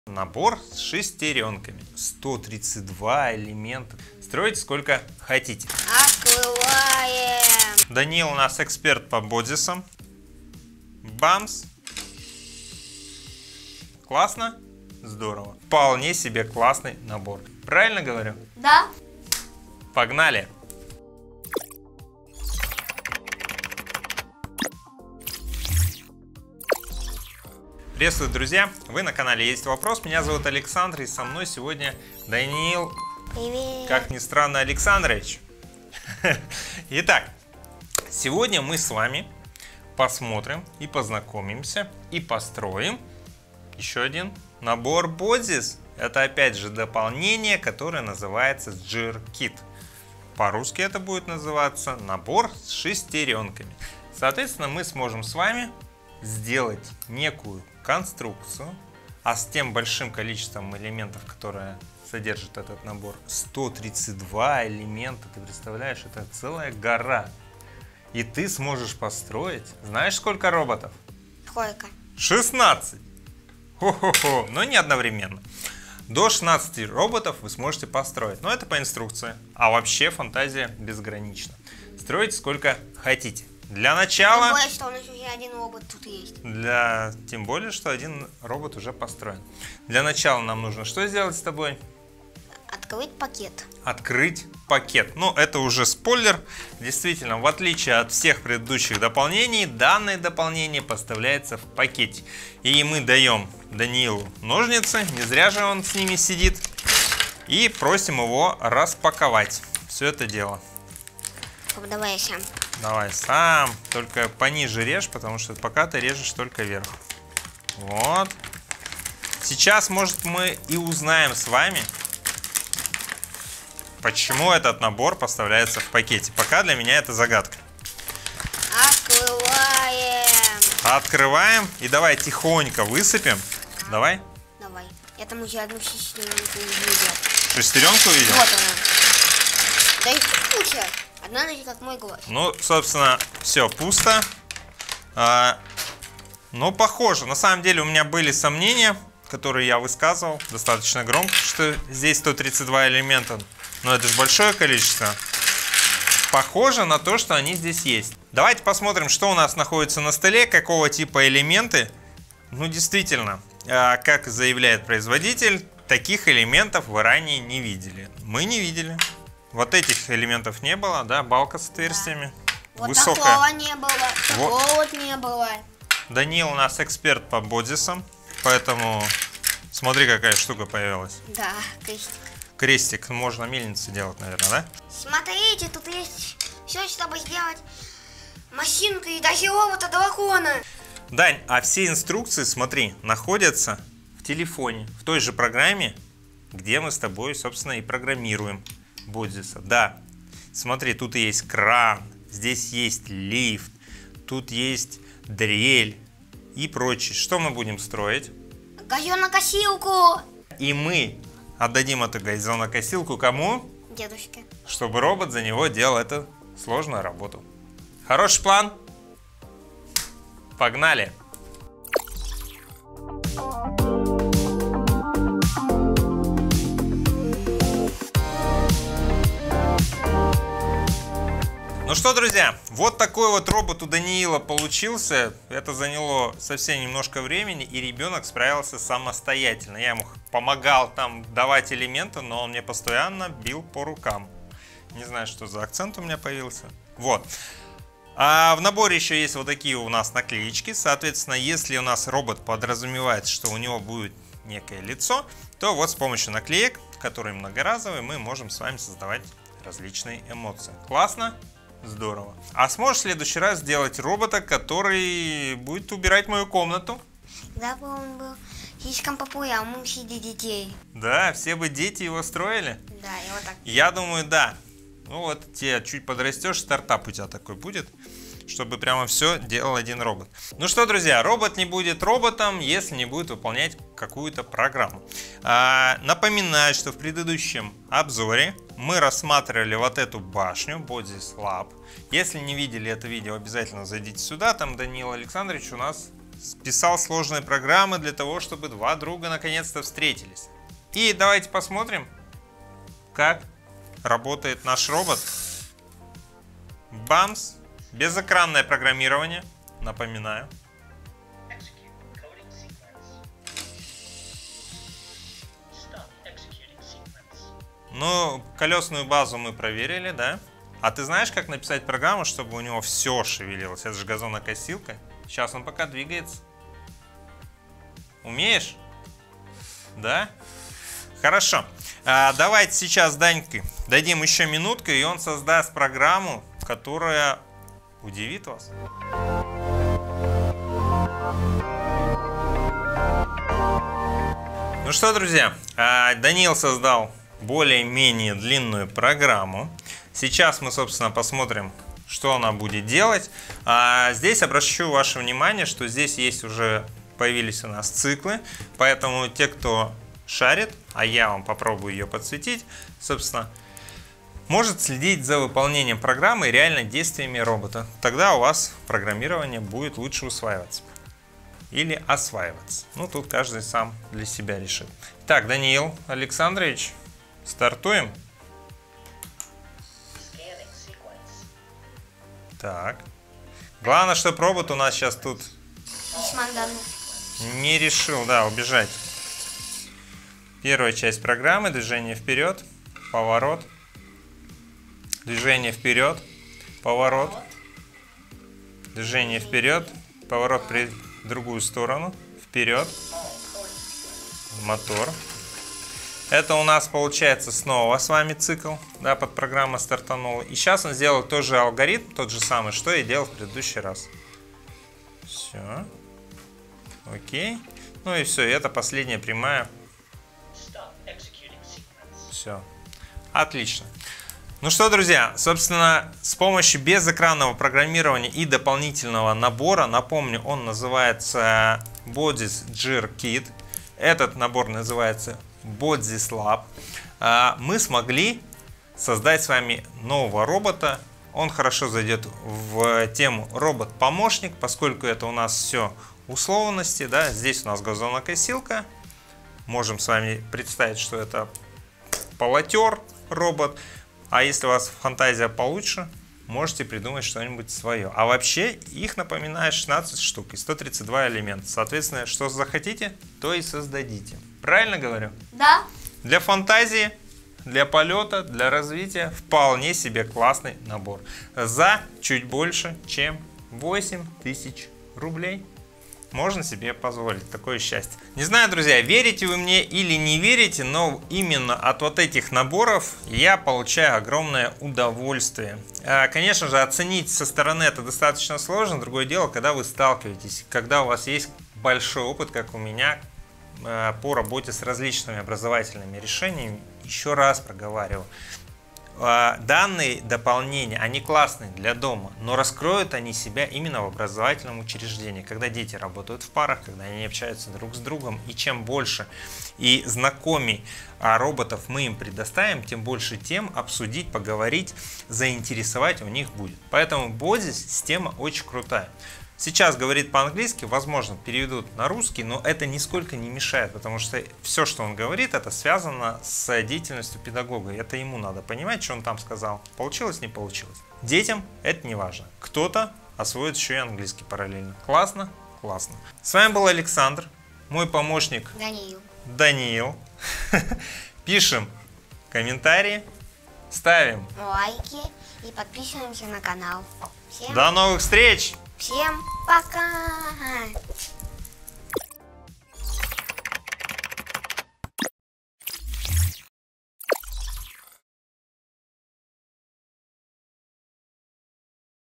Обзор BOTZEES GEAR KIT для Юных Инженеров | Конструирование в детском саду
Пусть ваши дети станут инженерами с BOTZEES GEAR KIT!

В этом видео мы расскажем вам о BOTZEES GEAR KIT — уникальном конструкторе, который идеально подходит для детского сада. Узнайте, как этот набор помогает развивать инженерные навыки, критическое мышление и творческий подход у детей. Набор содержит 132 уникальные детали и позволяет создать до 16 различных механизмов, что дает детям возможность изучать основы программирования и конструирования, а также виды передачи движения в механизмах.

Педагоги смогут использовать его для создания увлекательных и обучающих занятий, а дети — для веселого и полезного времяпрепровождения. Не упустите возможность увидеть, как просто и интересно можно учить малышей конструированию.

Приобретайте BOTZEES GEAR KIT и убедитесь в его возможностях сами!